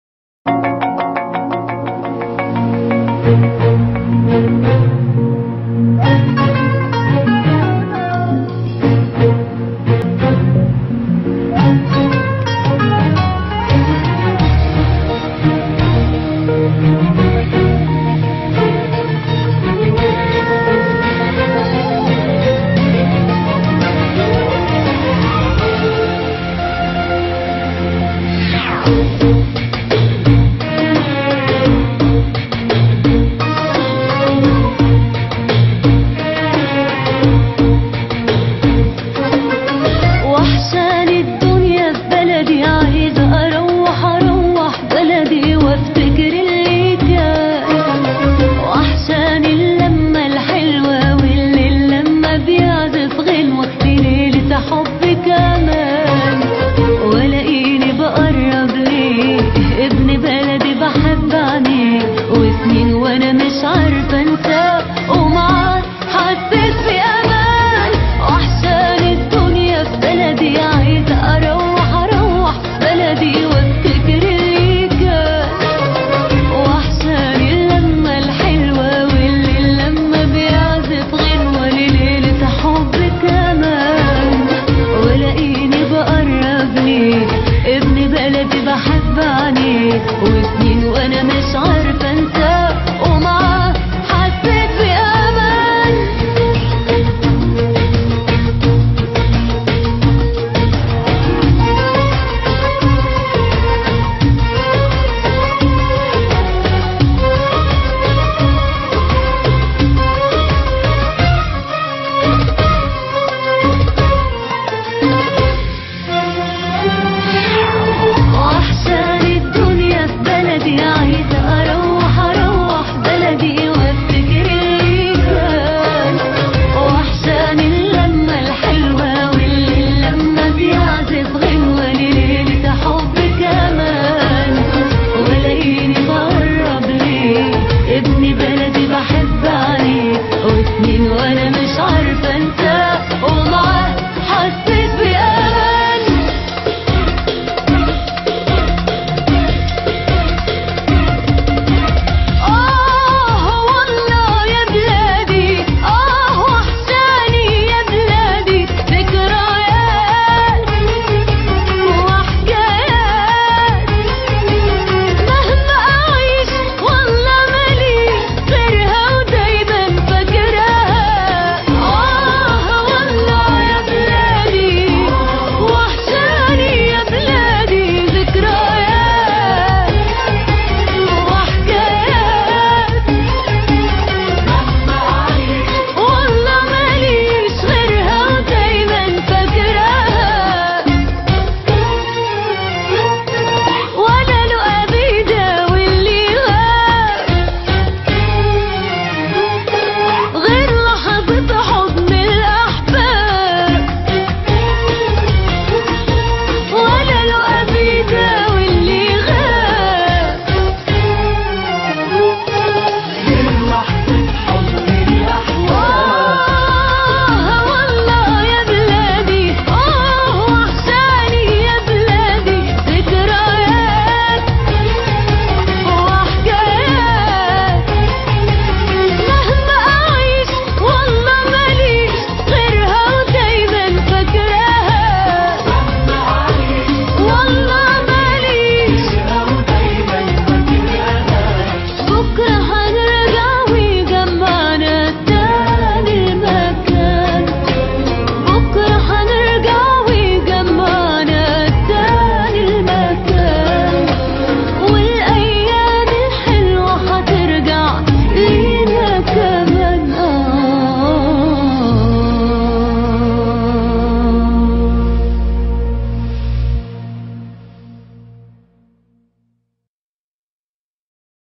Min film om barzeh وحشاني الدنيا ببلدي خاص لاهل برزة الغوالي
إهداء لكل أهل بلدتي الحبيبة برزة
لكل المغتربين بأجسادهم
و المغربين عن بيوتهم
يرونها بأعينهم و قلوبهم تتحسر
حزناً عليها
ترونها بأعينكم و نراها بقلوبنا
الله يفرجها عليك يا بلدي و يحميكي
و ترجعي أحلى مما كنتي
و نرجعلك
لنعمرها و نبنيها  من جديد
لاتنسوني من الدعاء